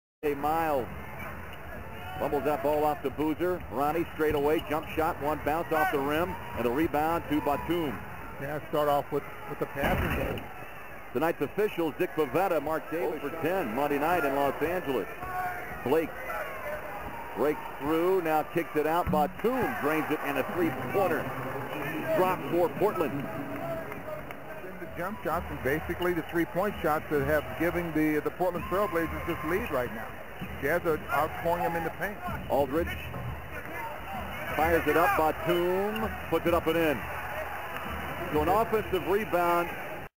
Nicolas Batum NBA Highlights Jazz vs Blazers 05th Nov 08
Nicolas Batum highlights against the Utah Jazz on November 05th 08. / Les meilleures actions de Nicolas Batum contre les Utaz Jazz le 05 Novembre 08.